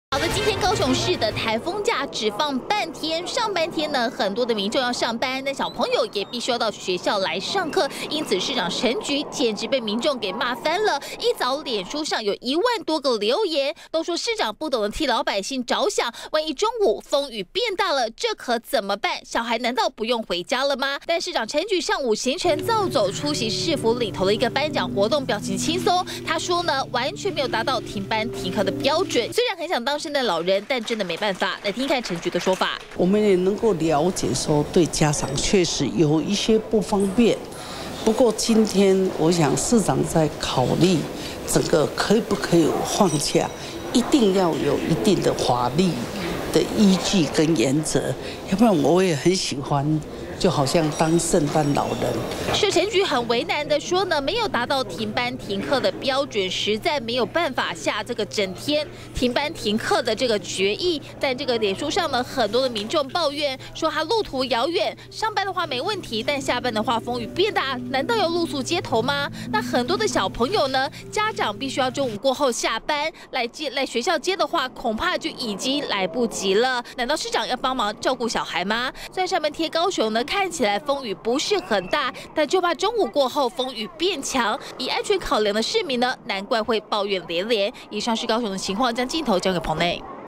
20160927中天新聞 半天颱風假被罵翻 陳菊：我也想當聖誕老人
高雄市上午還是要上班上課，很多家長罵翻，從昨晚開始市長陳菊臉書就被灌爆，但陳菊上午行程照走，詳情交給記者「高孟柔」。